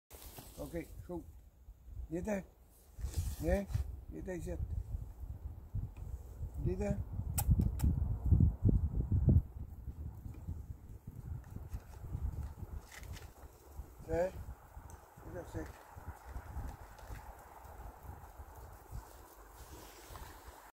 Michel et son écureuil
Apprivoisé un écureuil c'est très facile.